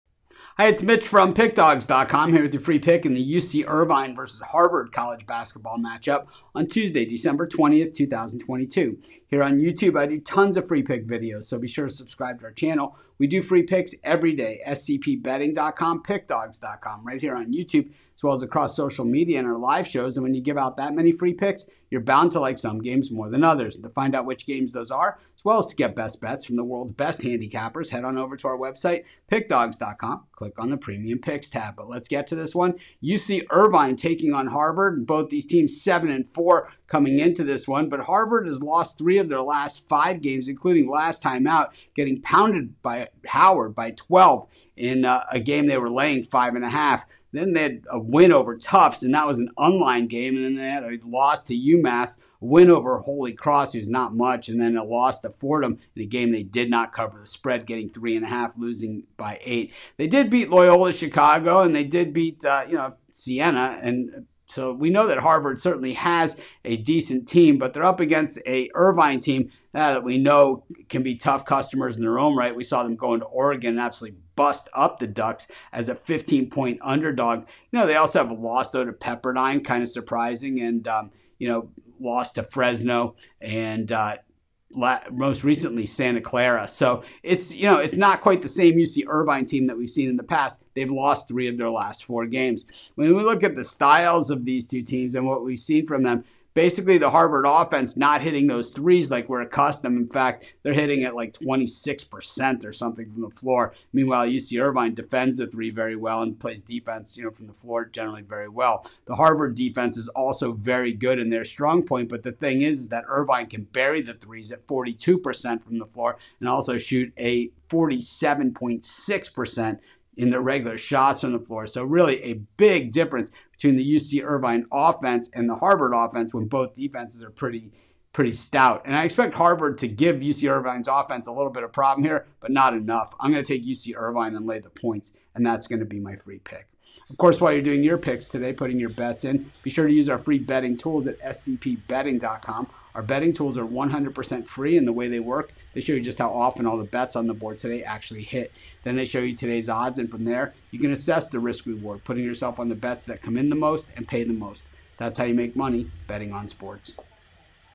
UC Irvine vs Harvard 12/20/22 College Basketball Free Pick CBB Betting Tips | NCAAB Picks
The Harvard Crimson take on the UC Irvine Anteaters in Tuesday College Basketball Action. Get Mitch's College Basketball Pick and Prediction for this game.